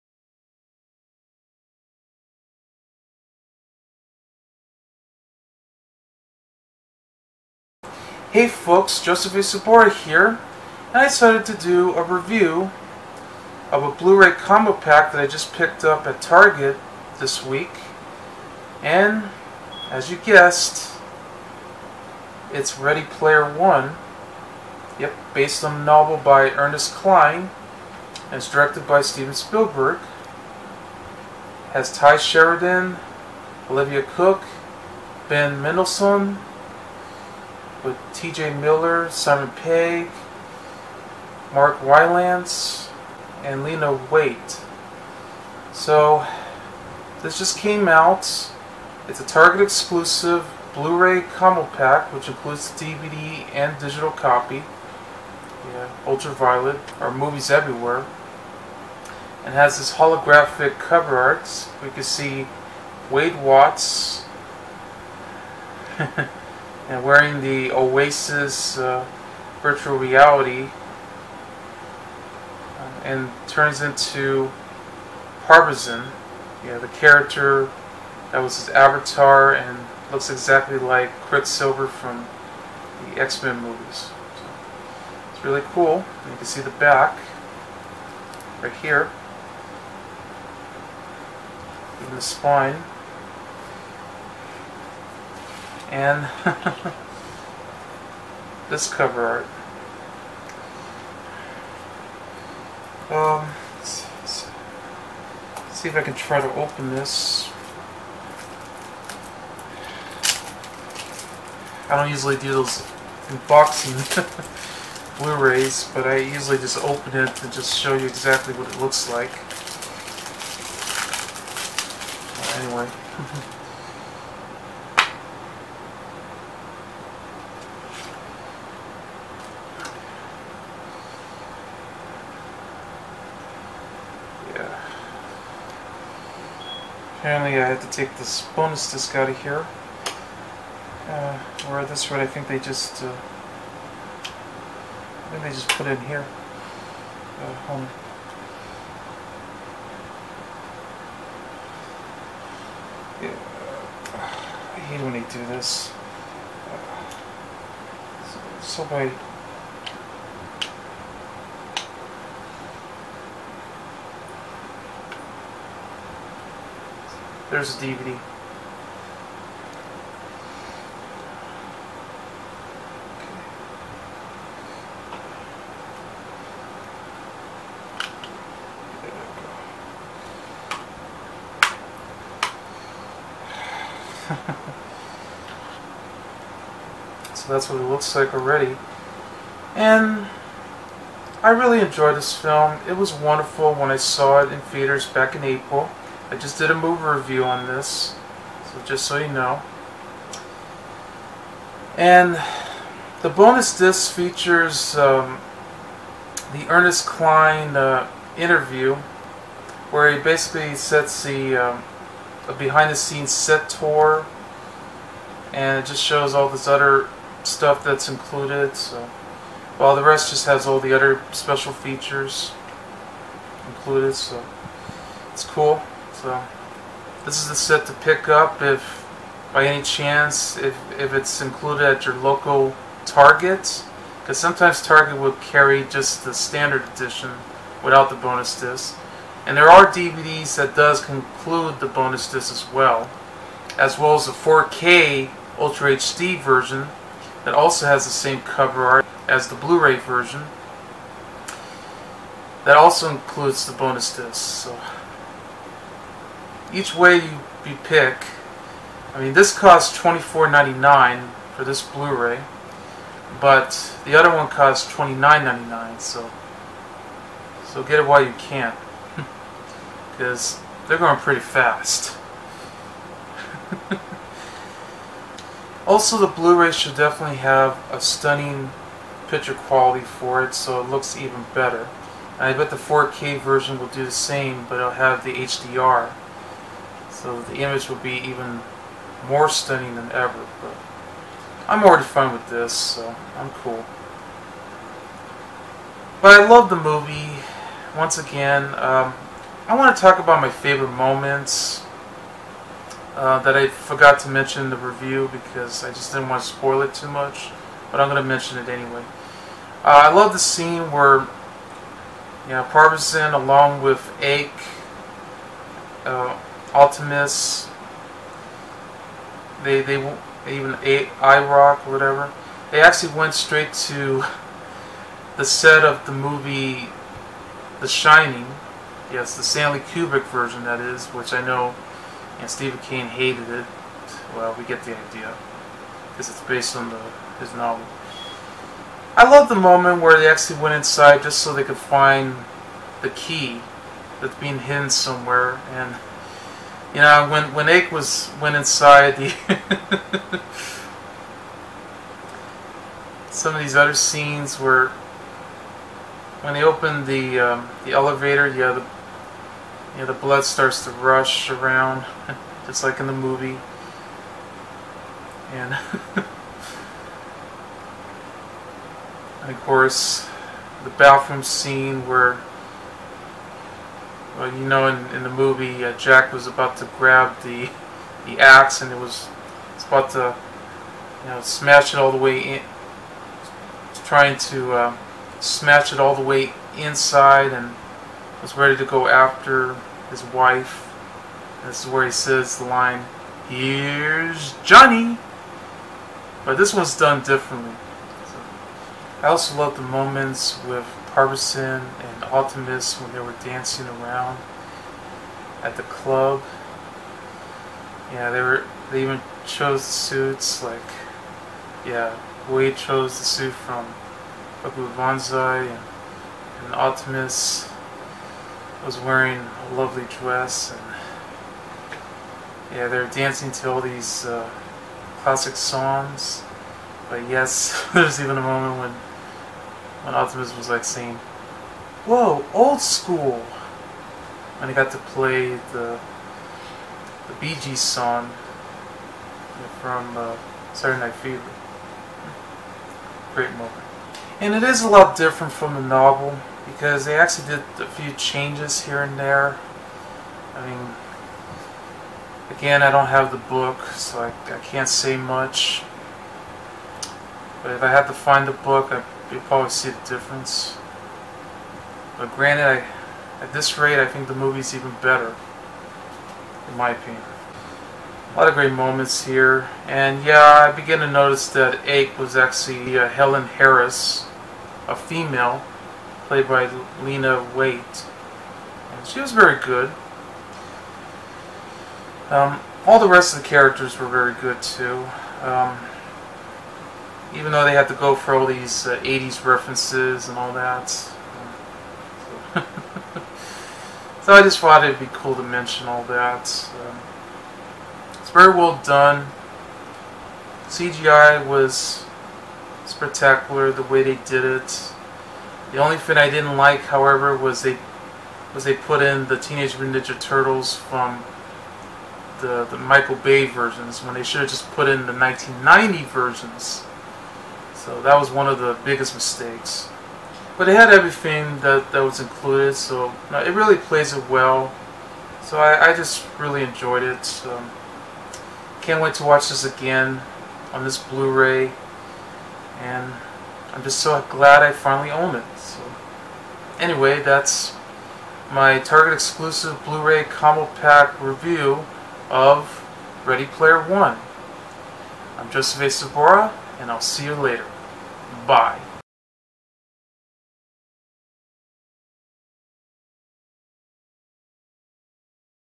Ready Player One (2018): Target Exclusive Blu-Ray Combo Pack Review
Doing a review of the Target Exclusive Blu-ray Combo Pack of "Ready Player One" (2018), just came out this week!